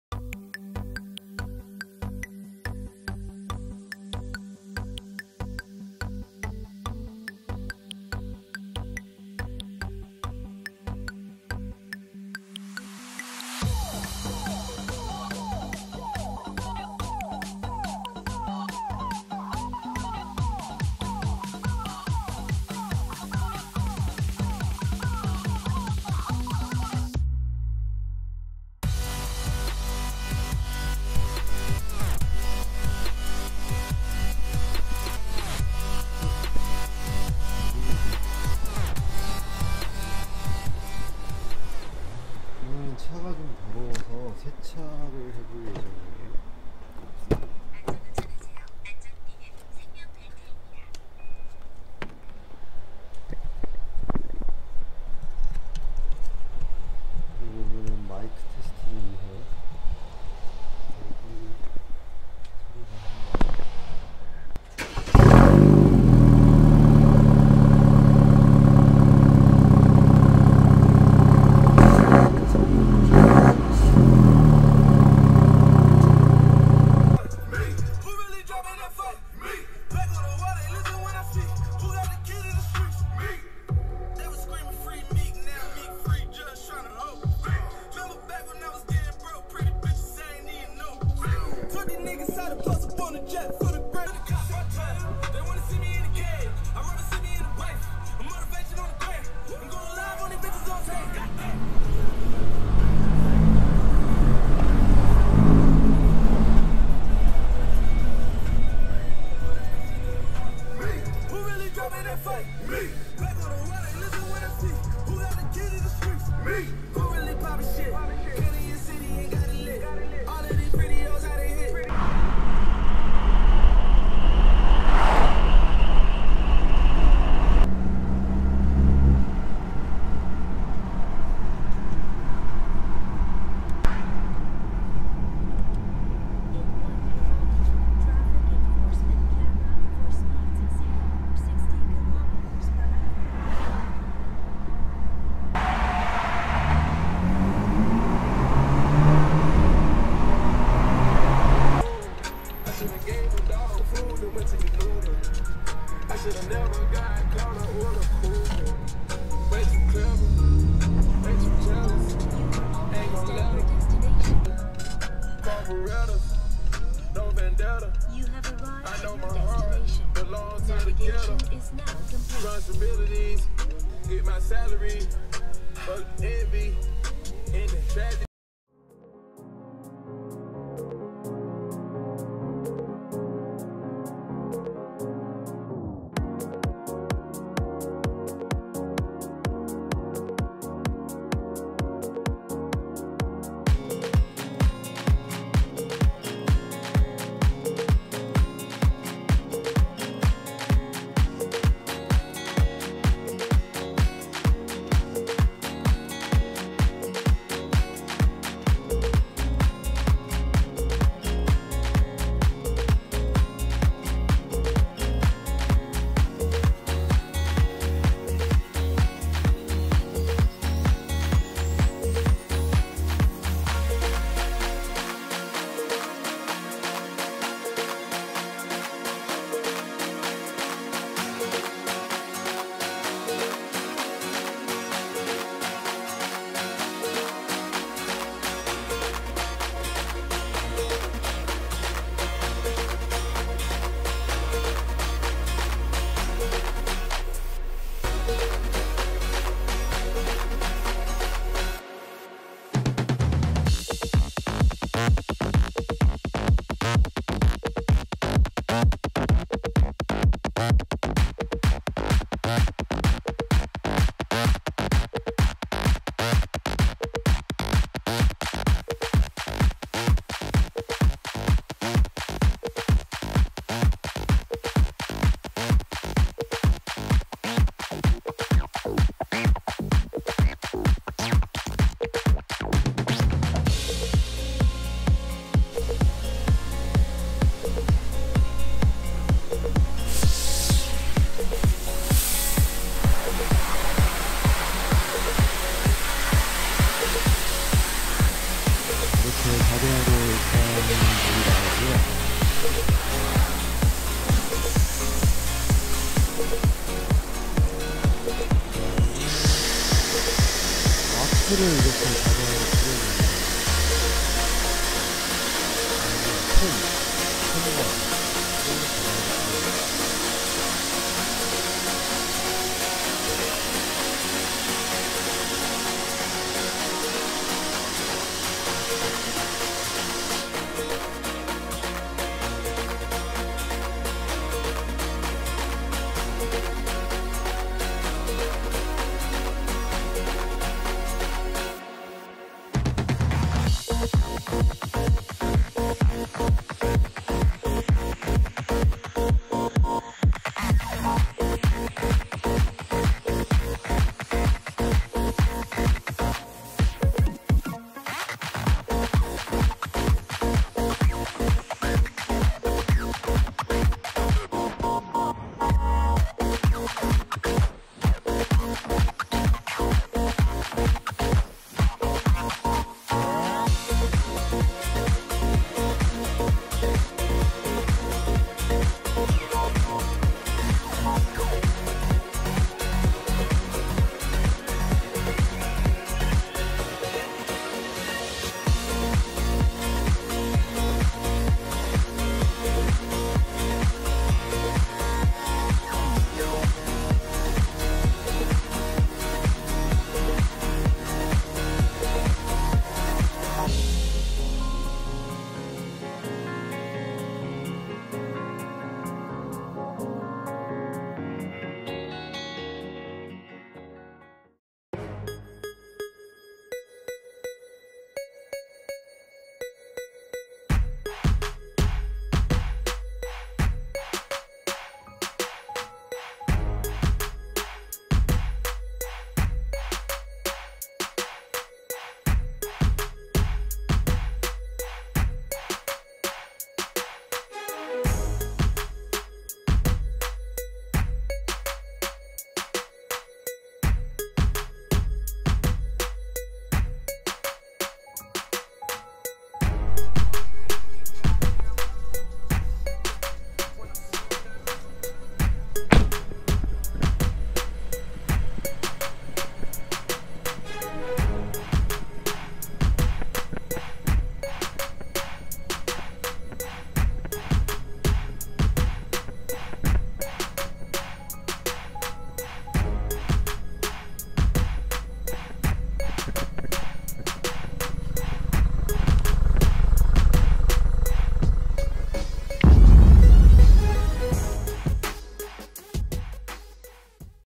SEASON2_EP5_CAR WASH_용인 K Wash 세차장
I have visited one of the newest car washing spot near my home.

용인에 새로 생긴 반자동?세차장을 방문하였습니다. K Wash라는 곳인데 사장님이 매우 친절한 곳입니다.

South Korea.
Shot with Gopro Hero 7
Edited by Whye$m